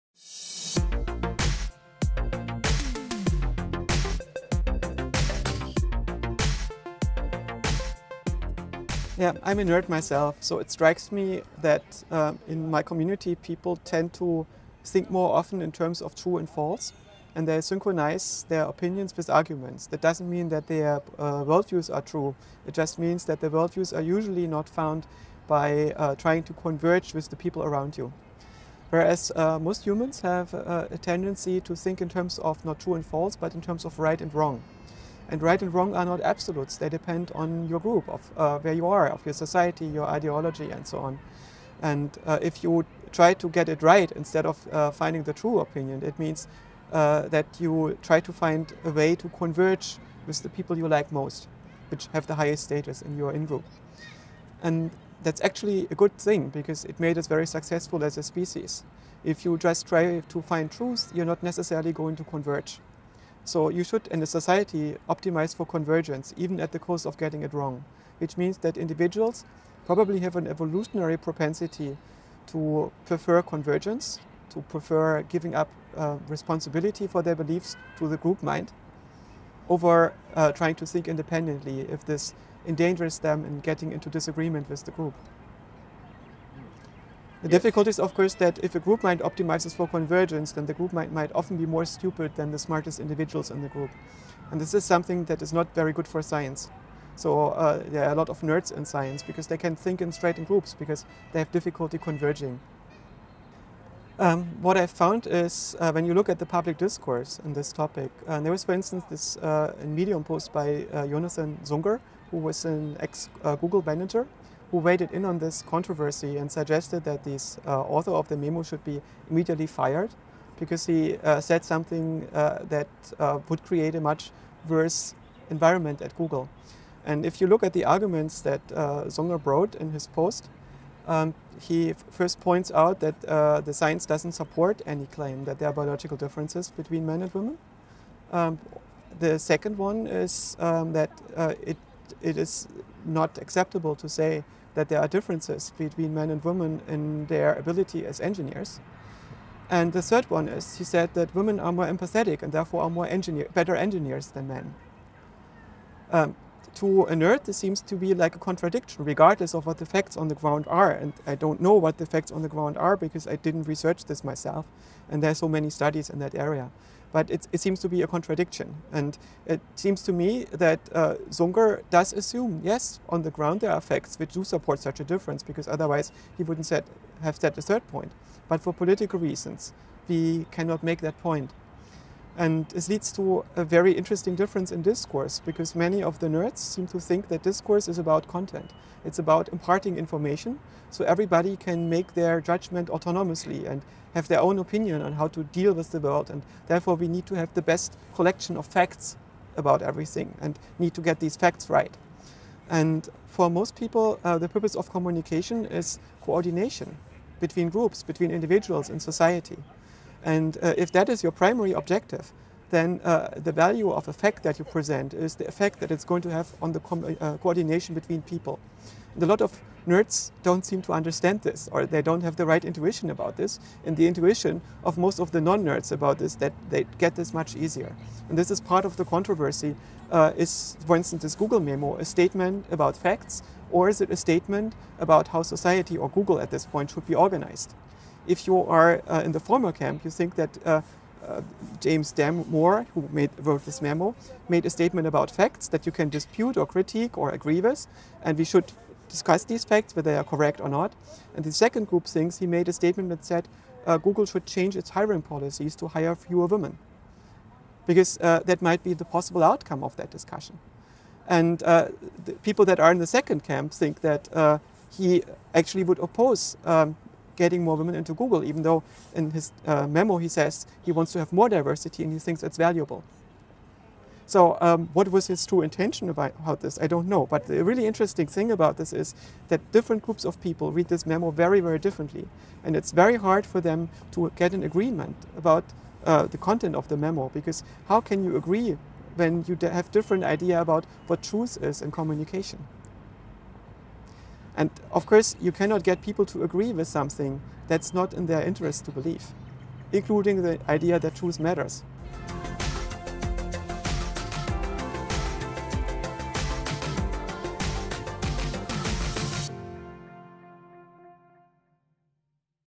Cognitive Biases & in-group convergences - Joscha Bach
Discussion points:
- In-group convergence: thinking in true & false vs right & wrong
- The group mind may be more stupid than the smartest individuals in the group

Joscha Bach, Ph.D. is an AI researcher who worked and published about cognitive architectures, mental representation, emotion, social modeling, and multi-agent systems. He earned his Ph.D. in cognitive science from the University of Osnabrück, Germany, and has built computational models of motivated decision making, perception, categorization, and concept-formation. He is especially interested in the philosophy of AI and in the augmentation of the human mind.

Joscha has taught computer science, AI, and cognitive science at the Humboldt-University of Berlin and the Institute for Cognitive Science at Osnabrück.

Follow Joscha Bach:
bach.ai
liquid.ai

b) Donating
- Bitcoin: 1BxusYmpynJsH4i8681aBuw9ZTxbKoUi22
- Etherium: 0xd46a6e88c4fe179d04464caf42626d0c9cab1c6b

Kind regards,
Adam Ford
- Science, Technology & the Future